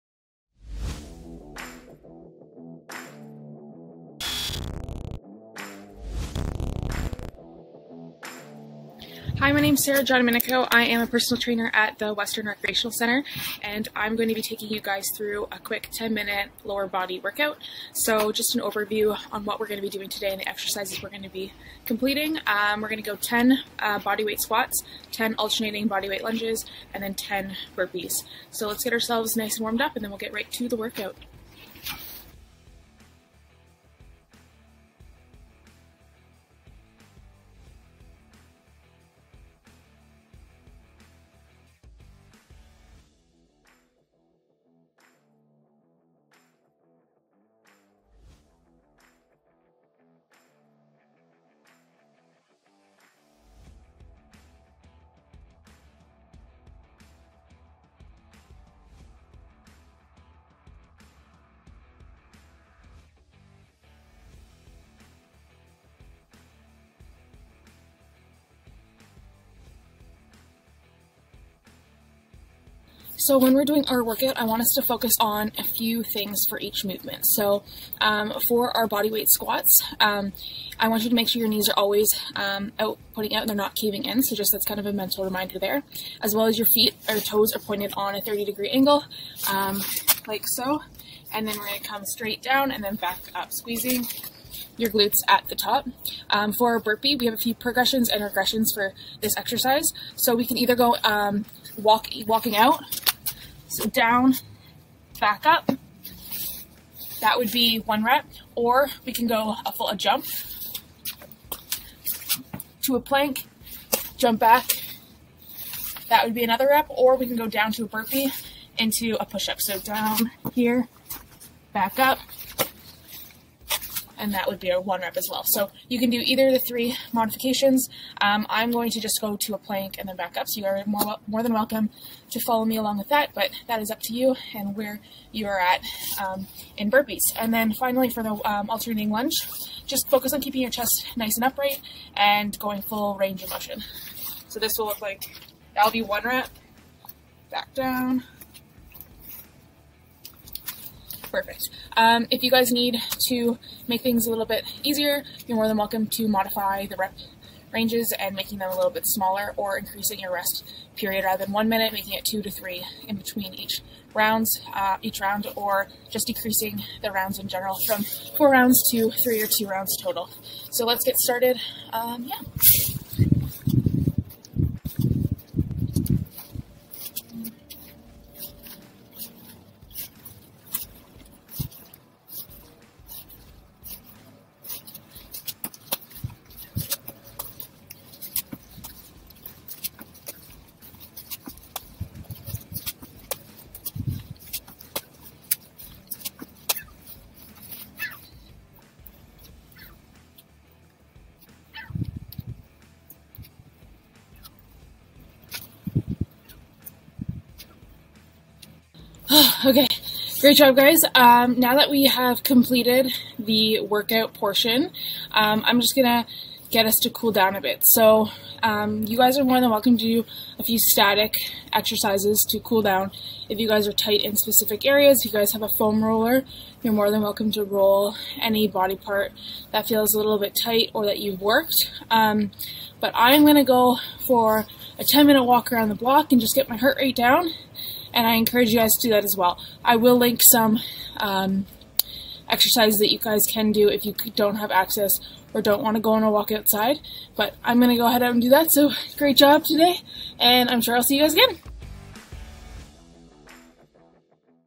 Mustangs Trainer Tuesdays: Quick Lower Body Workout
Western Campus Rec Trainer Tuesday: Quick Lower Body Workout with Sarah

Sarah our Personal Trainer at the Western University's Recreation Centre is back again this week, to walk us through this speedy 4 minute workout video. All you really need to follow along is some space to do the exercises shown and your body. Remember to pay attention to the modifications and feel free to shake it up in order to increase or decrease the difficulty level.

Sarah starts off this video with a quick warmup consisting of: 20 Alternating Calf Stretches, 10 Open the Gates, 10 Close the Gates, 10 Arm Circles in each direction, 10 Knee hugs, 5 Squats, and 5 Burpees. Next she moves into the main workout out meant to exercise your lower body which contains: 4 rounds of 10 Air Squats, 10 Alternating Lunges, and 10 Burpees.

20 Straight Arm Push-ups, 40 Russian Twists (20/arm), all done x 3, and then resting for 1-1.5 minutes between round. For part 2 of this workout video, Sarah will be showing how to continue with: 10 Leg Raises, 10 Scissor Kicks, 10 Superman's, all done x2, and 1 minute rests in between each round. For our cool down today, Sarah again emphasizes the importance in doing our own exercises but does have some for us today! These include Couch Stretches, Down dogs, Down Dog to Calf Stretches, all done x3 each. A walk around the block is also fine but remember to continue social distancing! Remember to stay hydrated and stay safe.

This video is presented by Western University Campus Recreation.